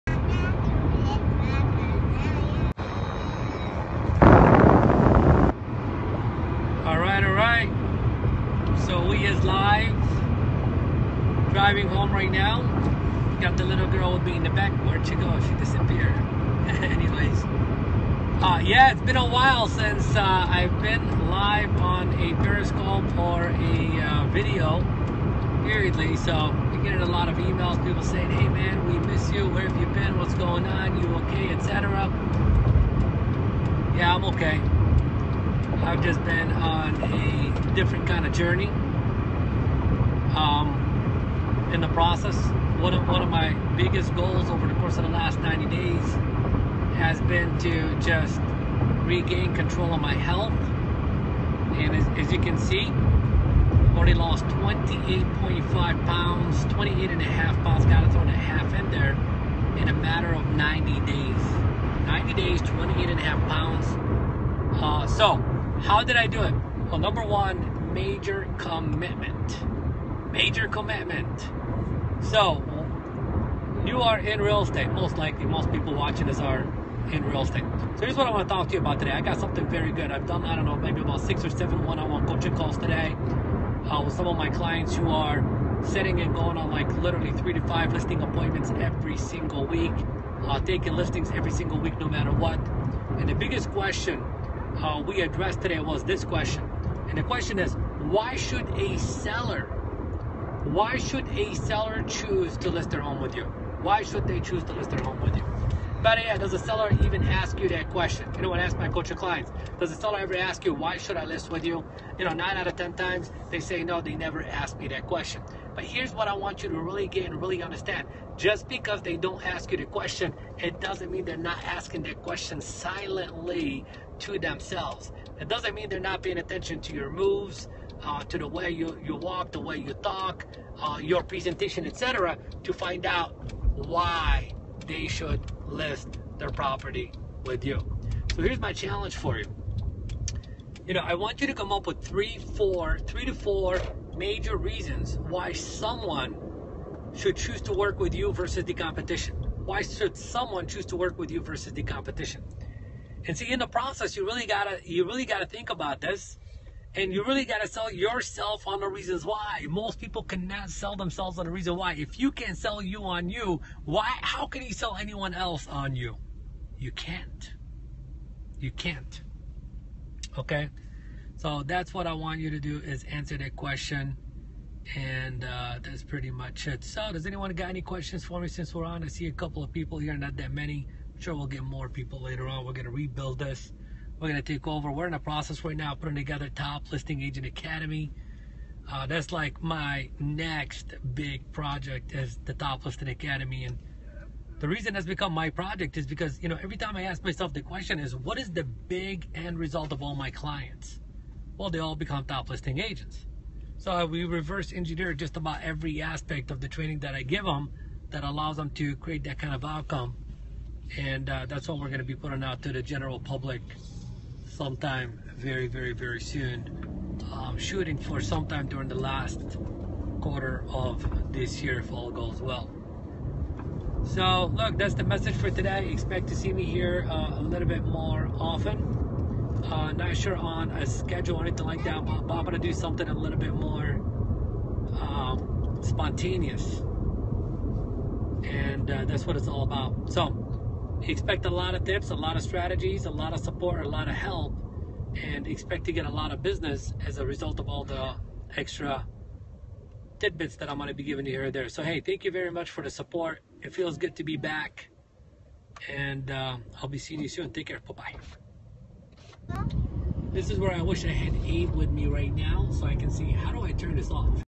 Why Should A Seller List Their Home with you?
Real estate listing presentation and handling objections.